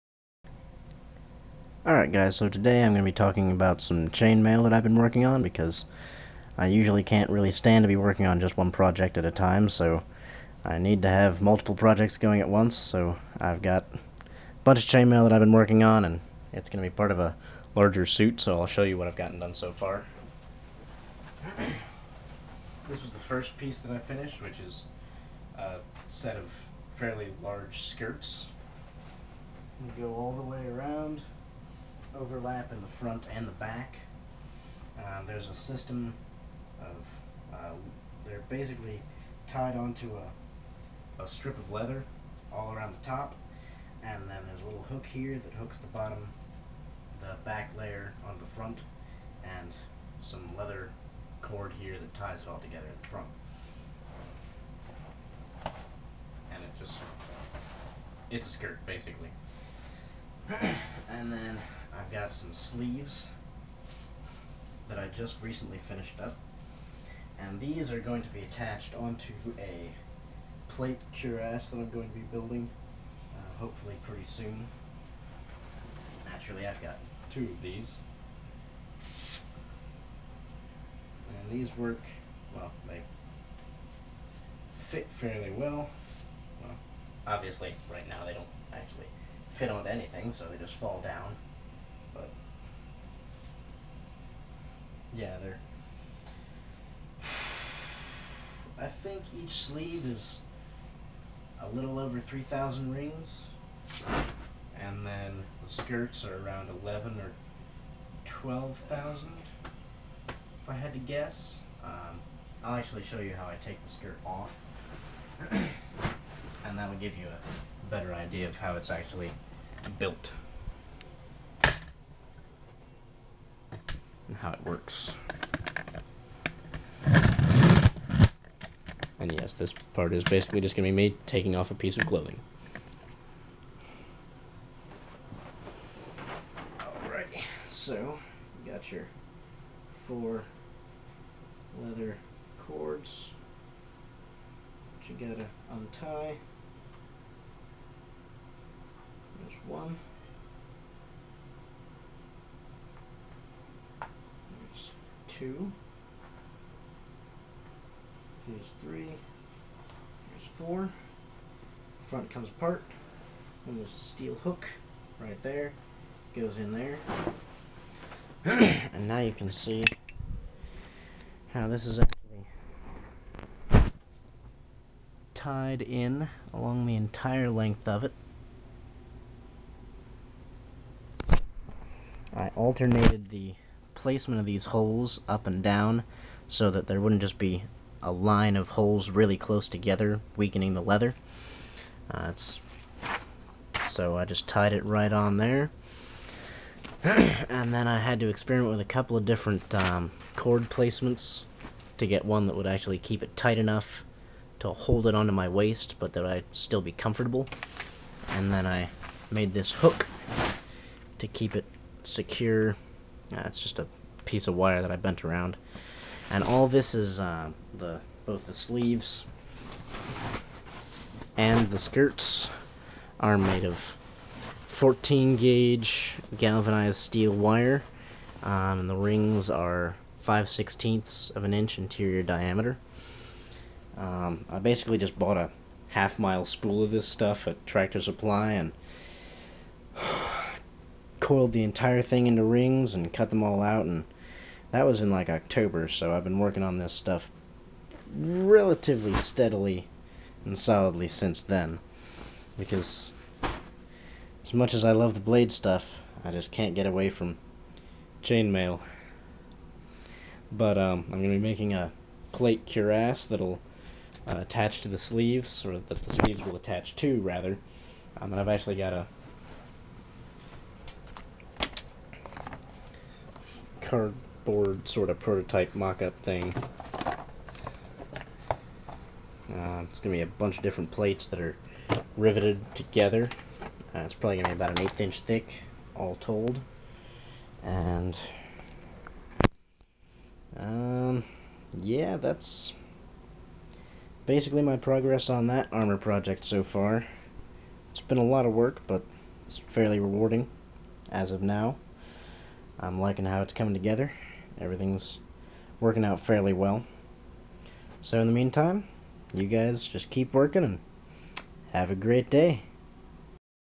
Armor Project in Process
Shortly after making this video, I actually changed my mind about the cuirass, and am opting instead to go with a scale vest, since I already have a bunch of scales started.